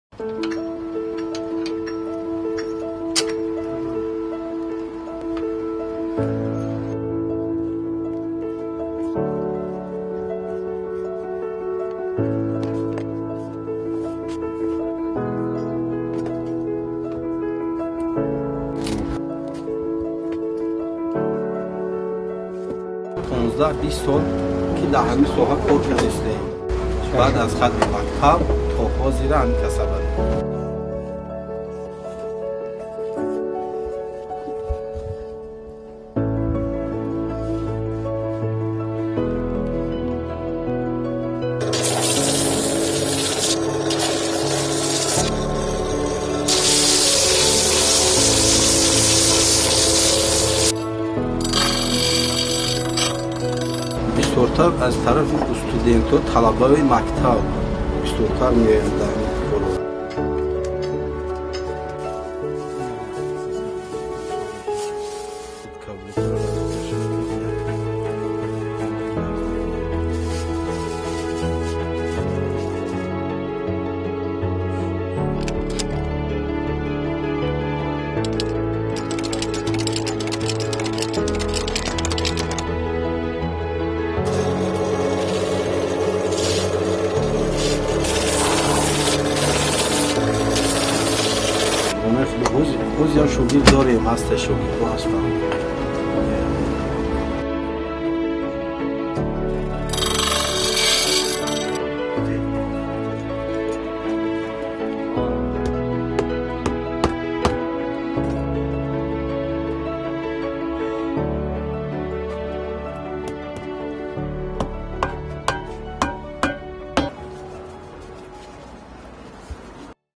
Чорабинии омӯзишӣ оид ба наворбардорӣ ва танзим
Чорабинии омӯзишии мазкур бо ташаббуси Интернюс дар Тоҷикистон ва ТҶ "Хома" рӯйи кор омад.

Машғулият дар доираи Барномаи расонаҳо дар Осиёи Марказӣ (CAMP) бо мусоидати Агентии ИМА оид ба рушди байналмилалӣ (USAID) ва Internews амалӣ мегардад.